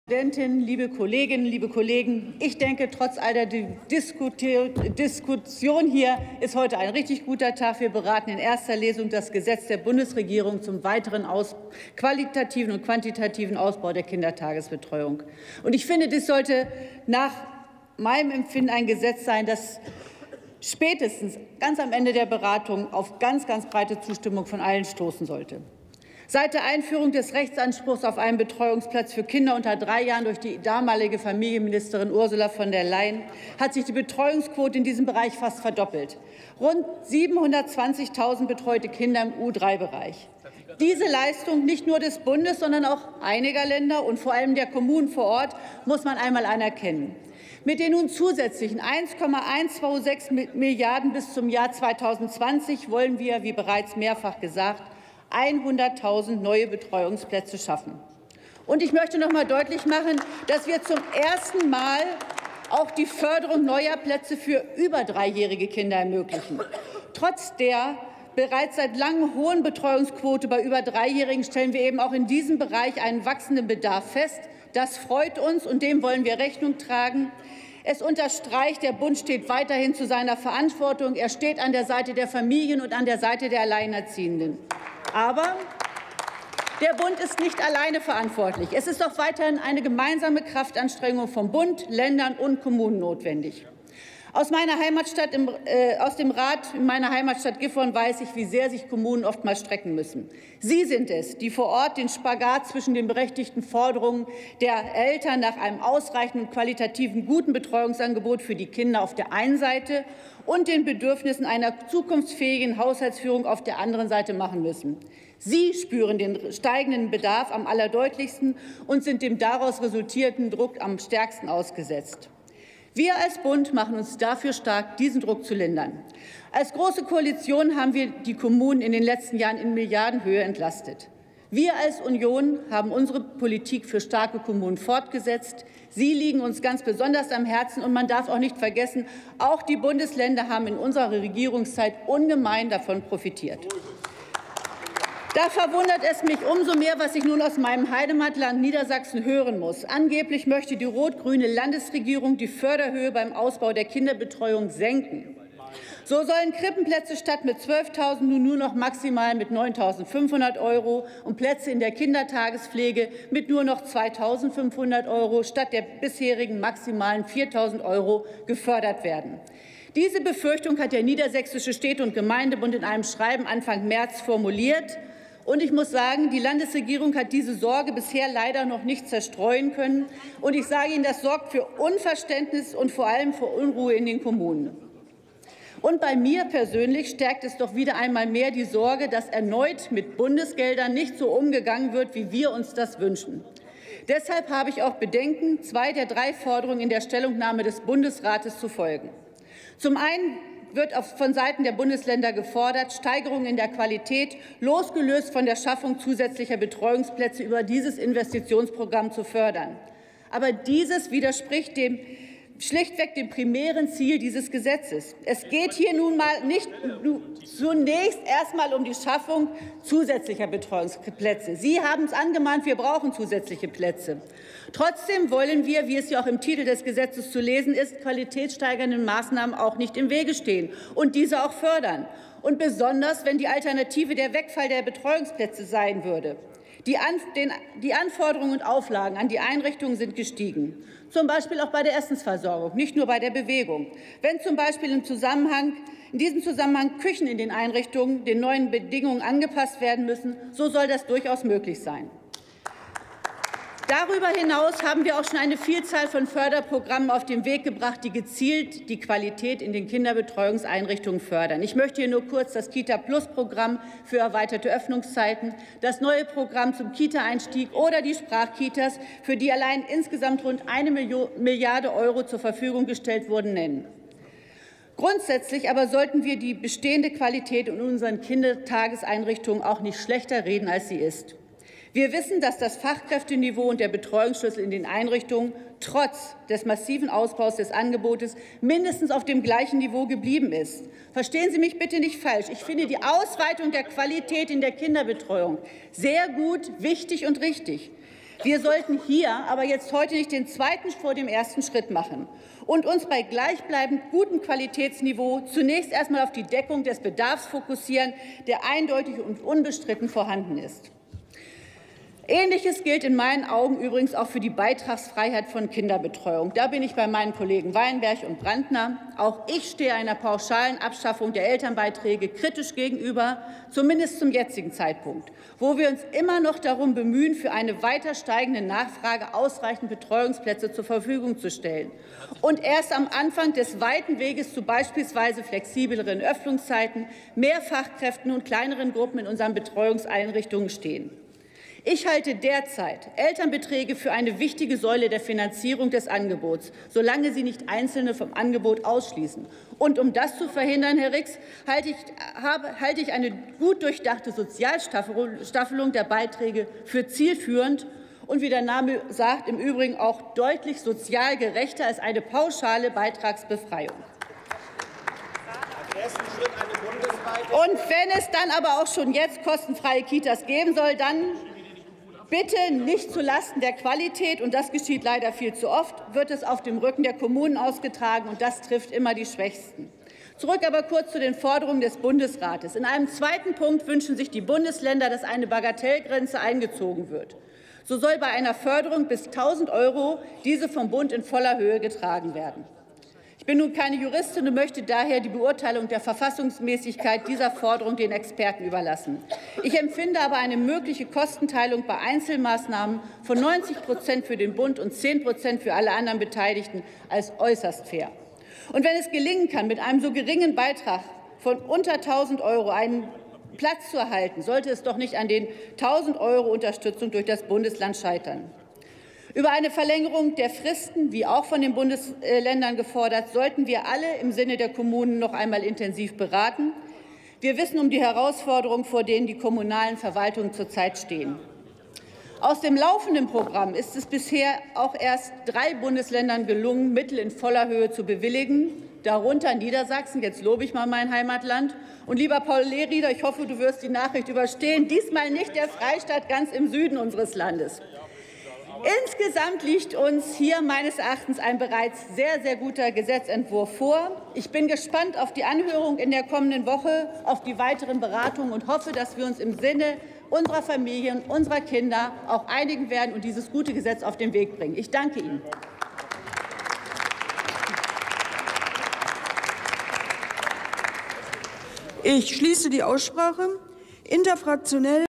Ingrid Pahlmann: Ausbau der Kindertagesbetreuung [Bundestag 23.03.2017]
Erste Beratung des von der Bundesregierung eingebrachten Entwurfs eines Gesetzes
zum weiteren quantitativen und qualitativen Ausbau der Kindertagesbetreuung
Drucksache 18/11408

18. Deutscher Bundestag
225. Plenarsitzung
Tagesordnungspunkt: Ausbau der Kindertagesbetreuung
Redner: Pahlmann, Ingrid, CDU/CSU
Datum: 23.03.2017
Plenarprotokoll:
UVID:7089843